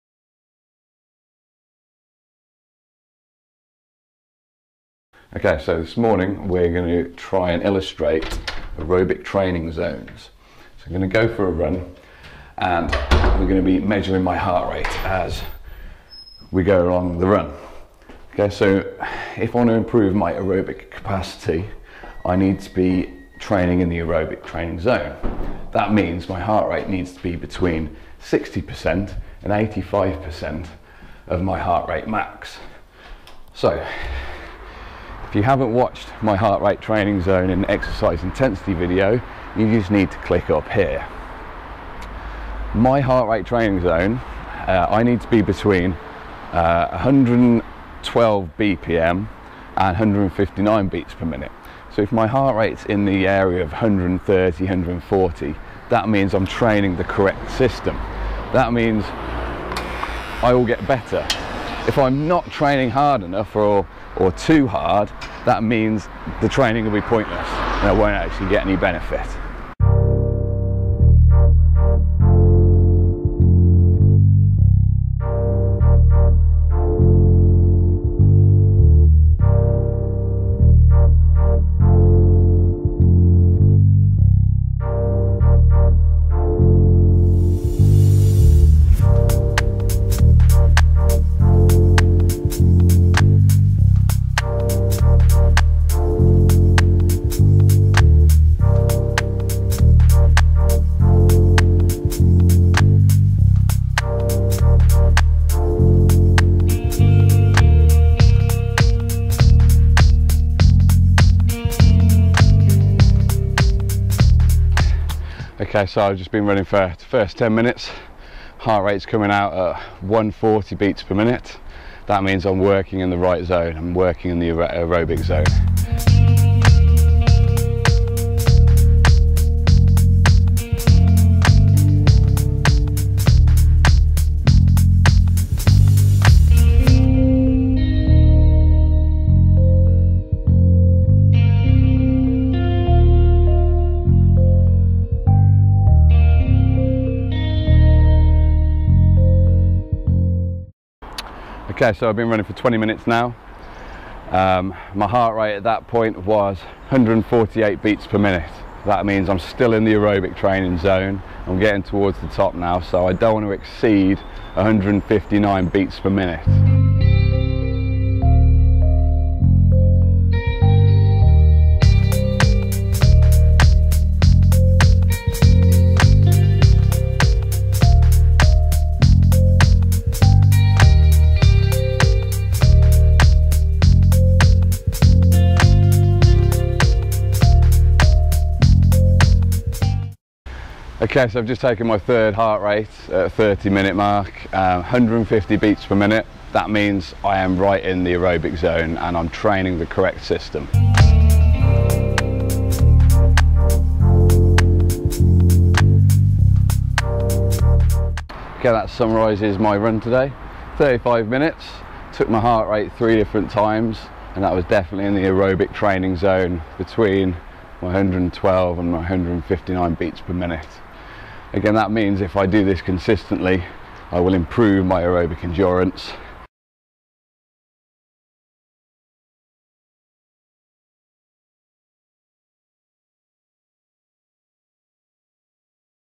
BTEC PE - Exercise Intensity: Heart Rate Training Zone Run
BTEC First in Sport
Unit 1: Fitness for Sport and Exercise

Exercise Intensity: Heart Rate Training Zone Run

Training run in the Aerobic zone:

60% - 112 bpm
85% - 159 bpm

Wearing:
MyProtein T-Shirt
MyProtein Panelled Sweatshorts